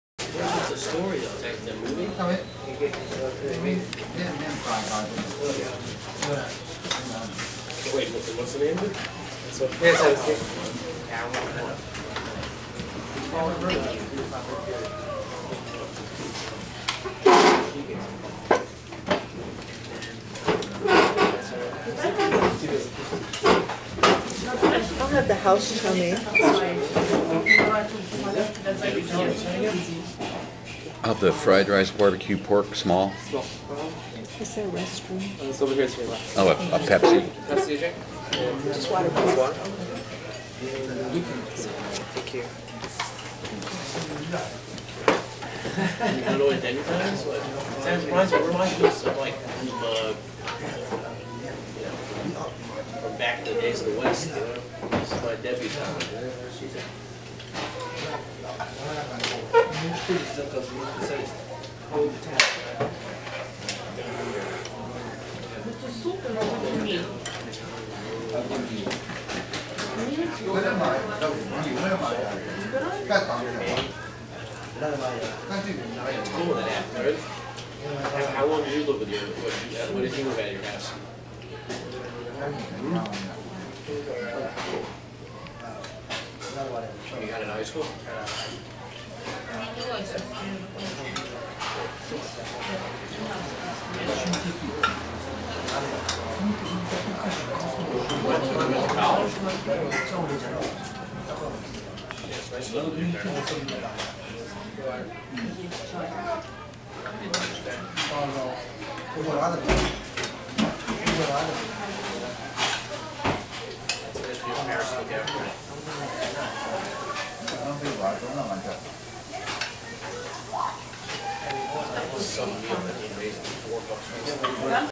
Sam Wo I in San Francisco
Supposedly the best food in China Town!! This the second floor.  The funny looking thing with the rope is how they get the food from the first floor to the second and third floors; like a dumb waiter.  The dirty dishes go down too.  Order a coke: the guy hands you can and a tiny paper cup.